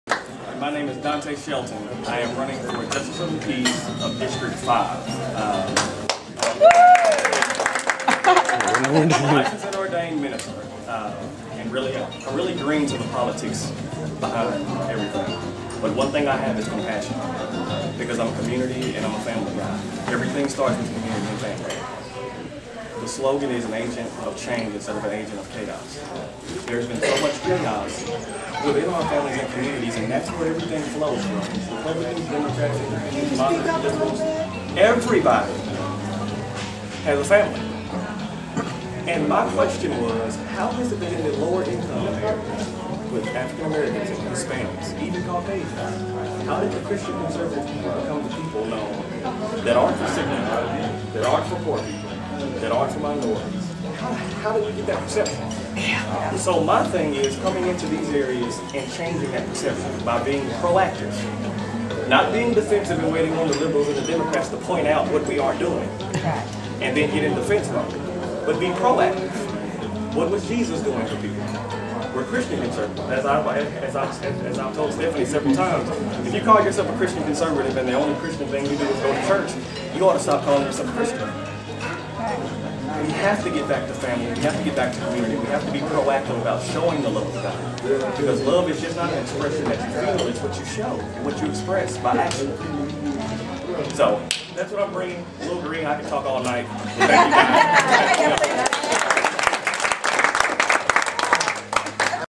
Dante Shelton for JP District 5 at Franke's Cafeteria.wmv
Dante Shelton talked about his candidacy for Pulaski County Justice of the Peace District 5. The 5th District is in Central Little Rock. You can learn more about Dante by visiting his facebook page Dante' N Katina  and name dlshelton.